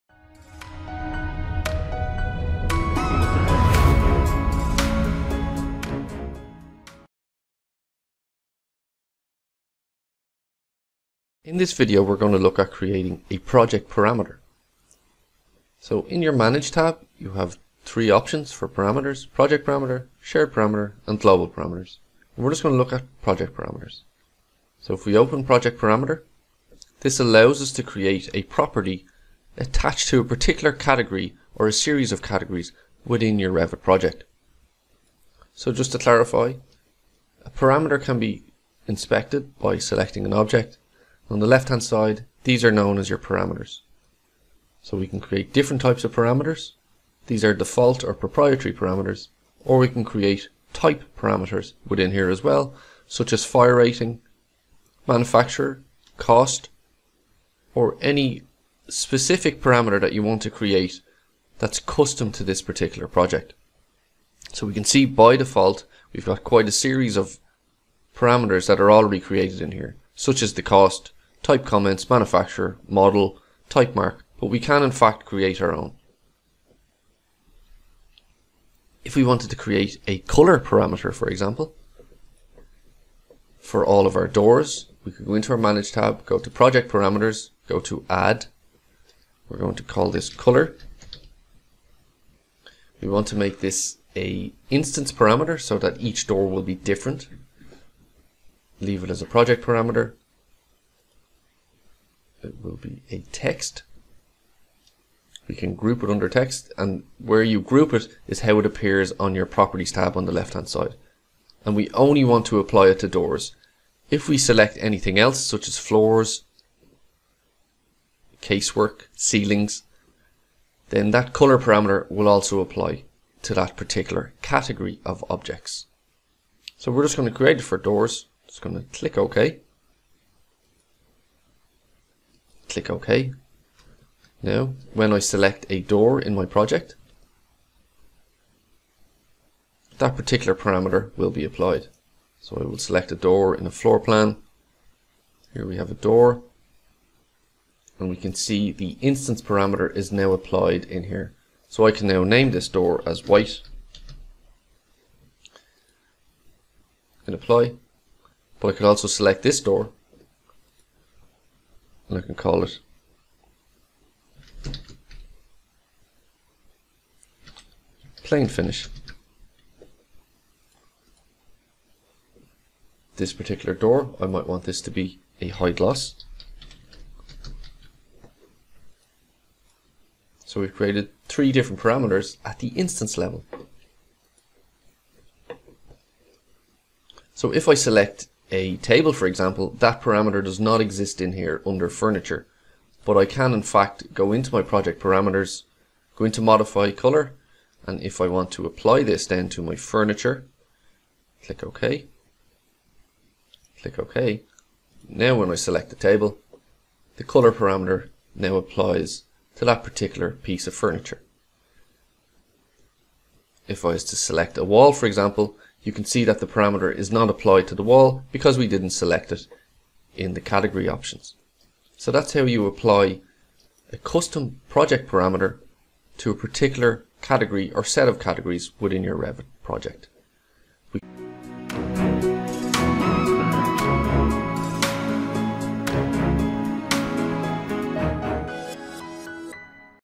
Autodesk Revit - Creating a custom project parameter
Project parameters allow you to apply custom properties to objects within a Revit project such as a colour parameter for a wall or door object. In this video we look at creating an instance parameter for door and furniture objects.